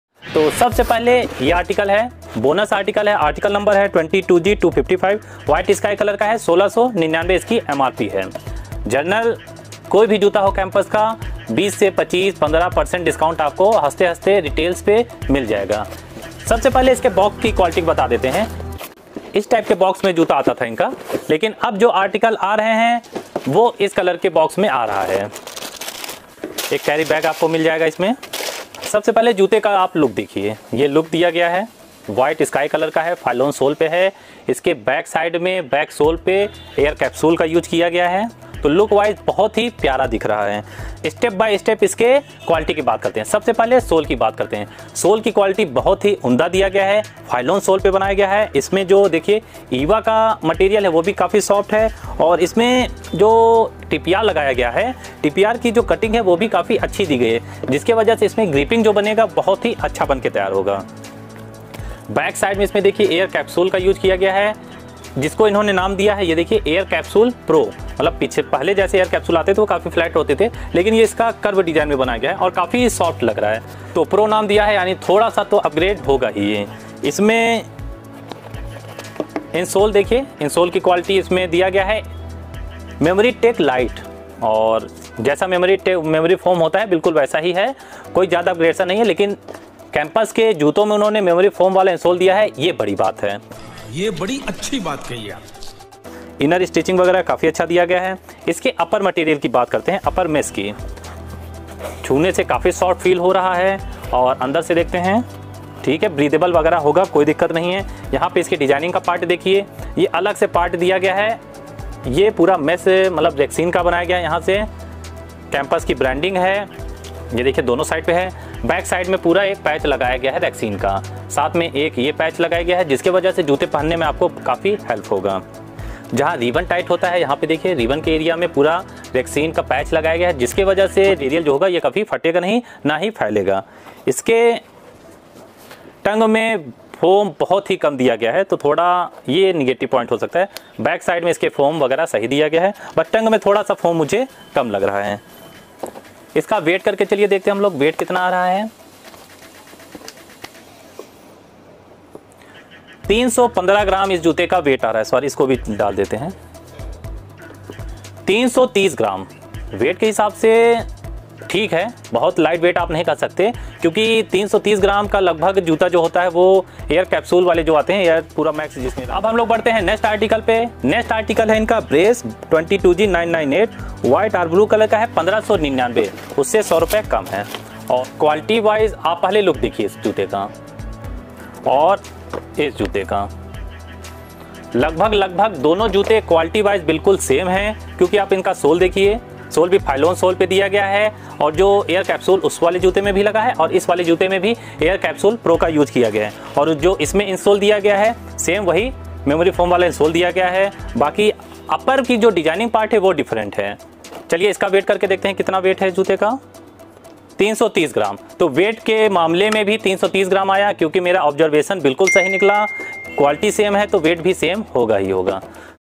Campus New shoes - Bonus and Brace - Campus shoes review - campus shoes new model 2023
In this video we are presenting you the latest campus shoes. In this video you will get detailed review with complete on point analysis of campus shoes.